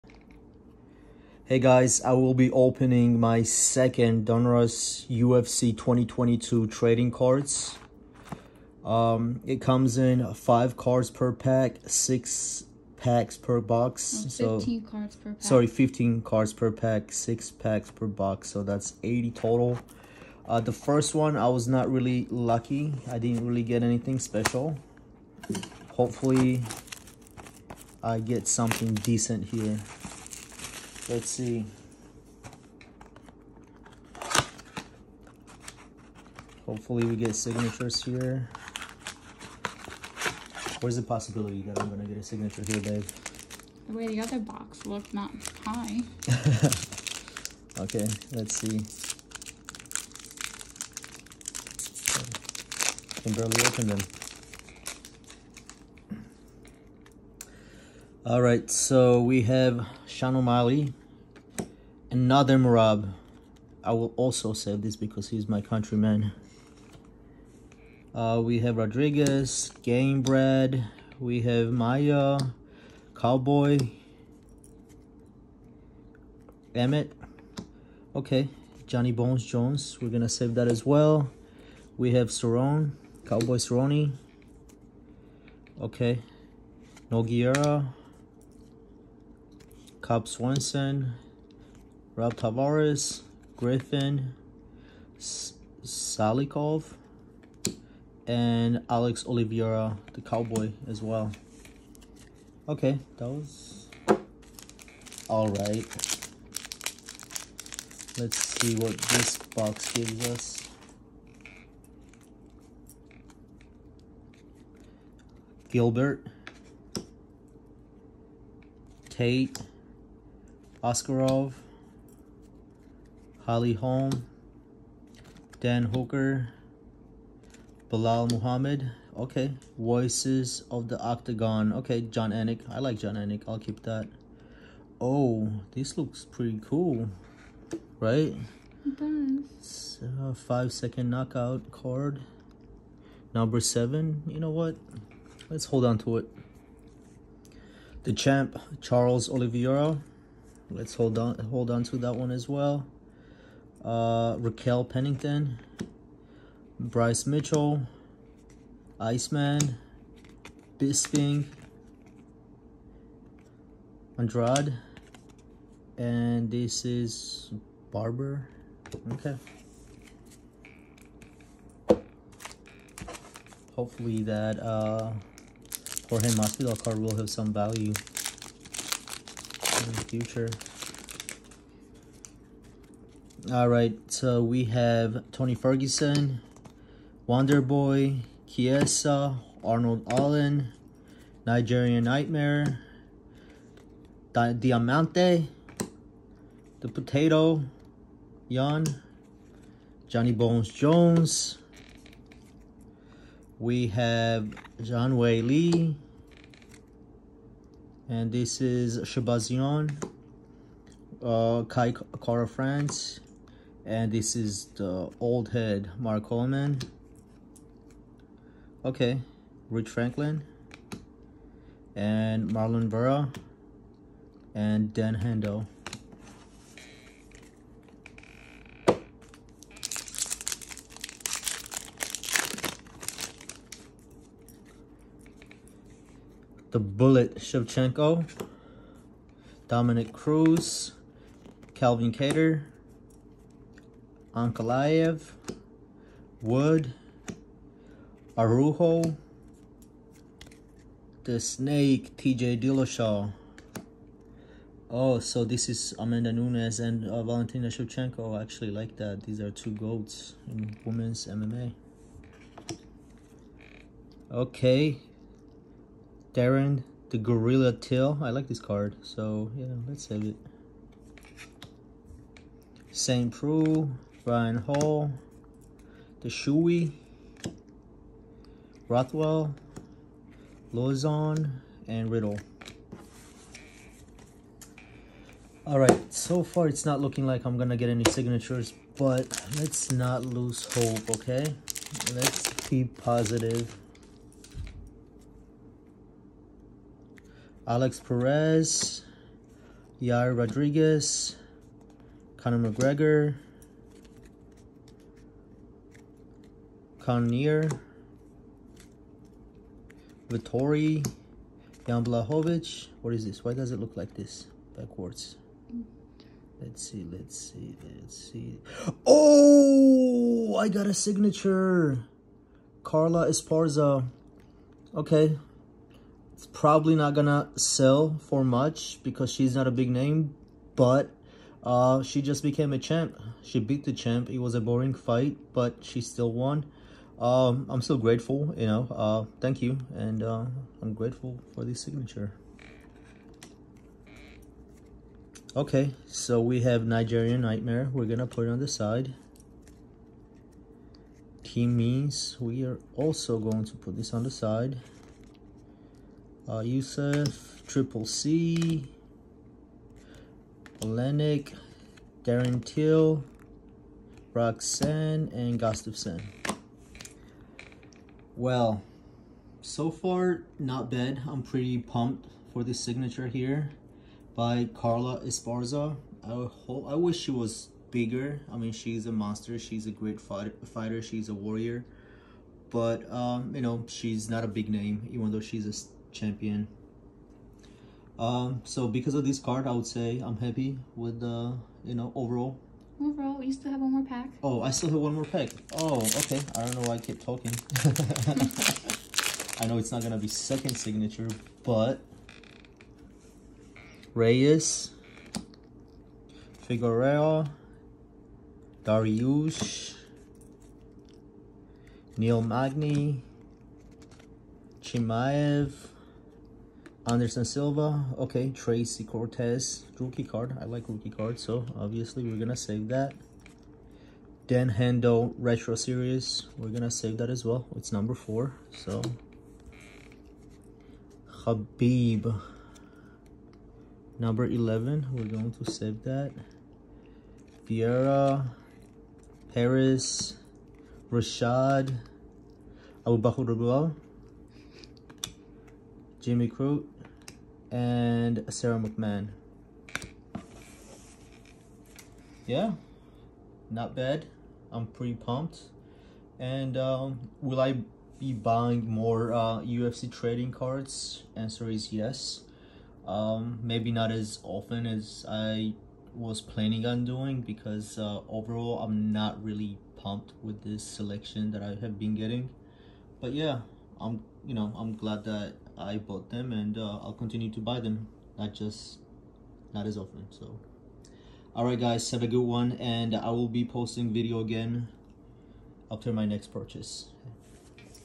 2022 Panini Dunruss UFC unboxing First Signature
Finally pulled a signature